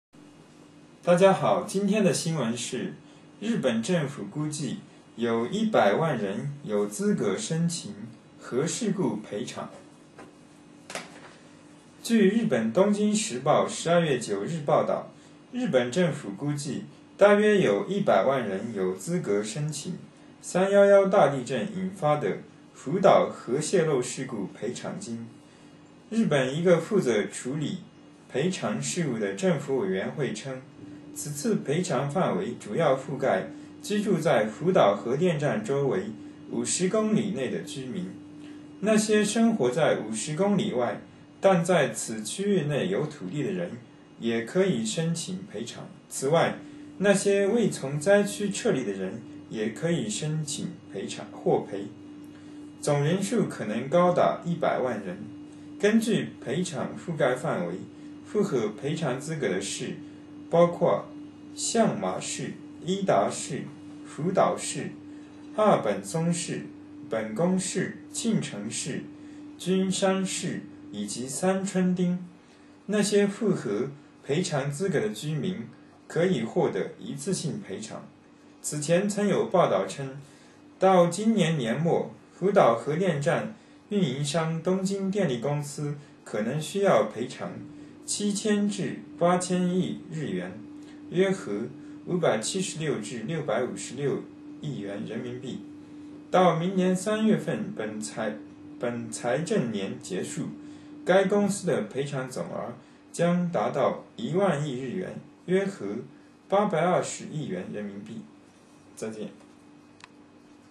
日本政府100万人が原子力事故賠償申請の資格があると予想
日中ニュース比べ読みサイトはこちらです。
中国語超特急講座はこちらです。